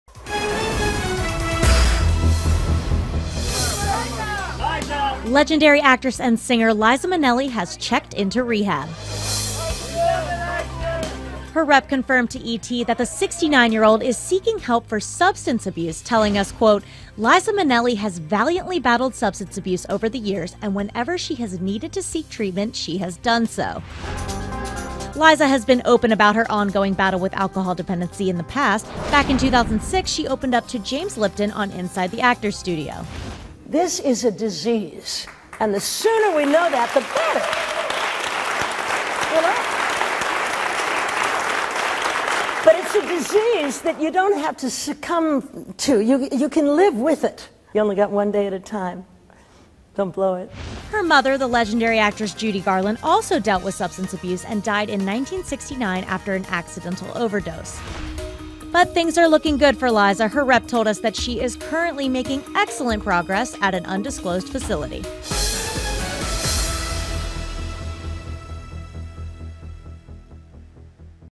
Liza Minnelli Checks Into Rehab
Liza Minelli is seeking help.